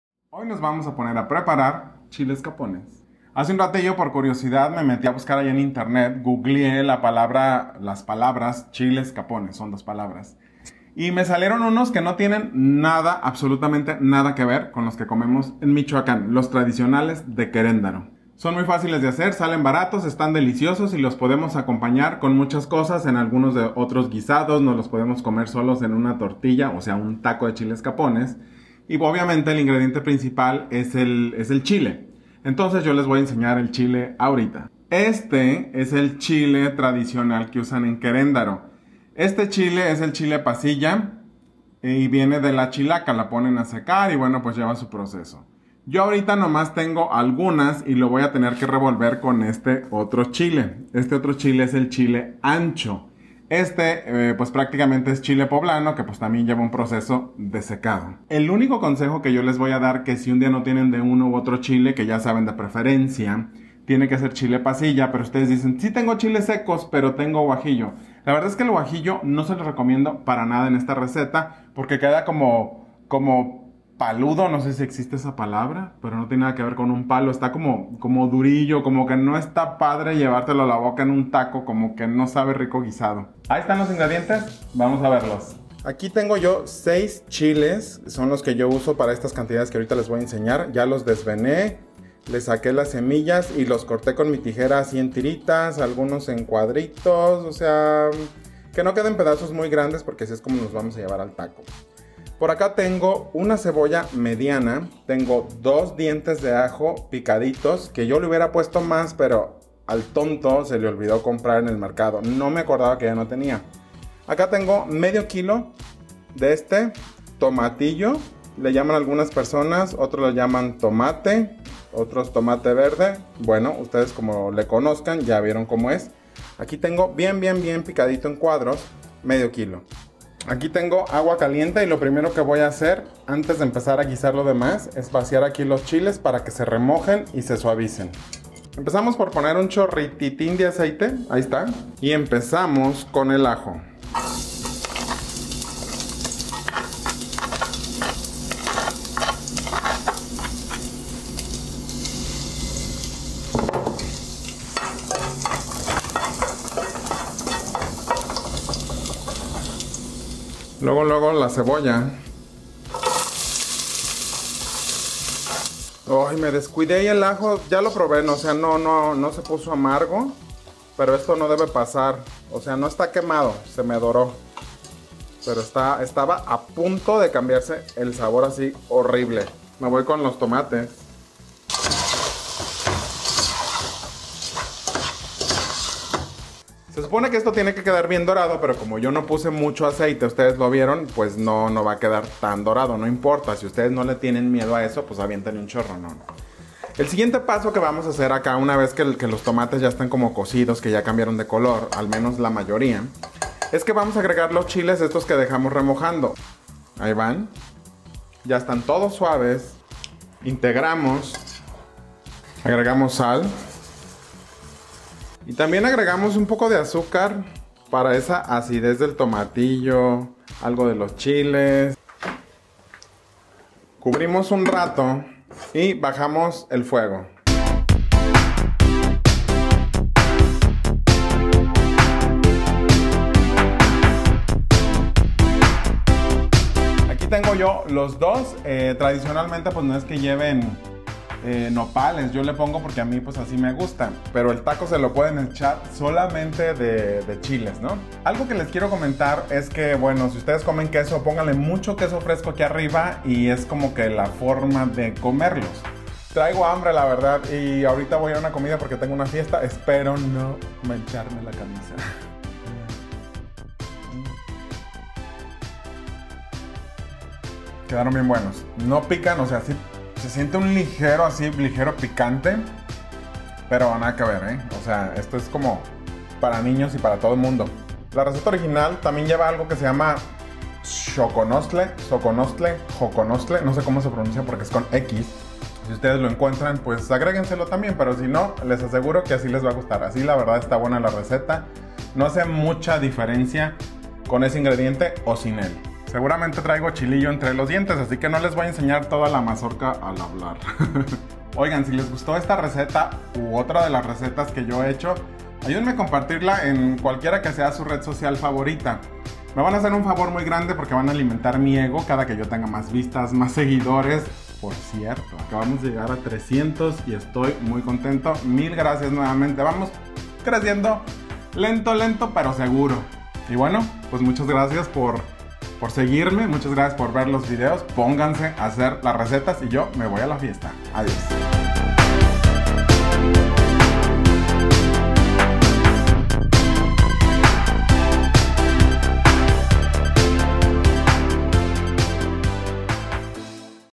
Chiles capones - Estilo Queréndaro, Michoacán (México)
Imagínense unos tacos de estos chiles con tortillas recién hechas... pues no esperen más y a darle. Esta es una receta casi (porque ya ven que les cambio ingredientes y formas de preparación a las recetas, por ejemplo en esta no pasé los chiles por aceite antes de agregarlos al tomate) tradicional. Si visitan Michoacán, dense una vuelta por Queréndaro; y si es en agosto, pues mucho mejor porque tienen la "Feria del Chile".

Si les gustó ayúdenme a compartir. Si no están suscritos, los invito a que lo hagan.

¡Gracias por pasar!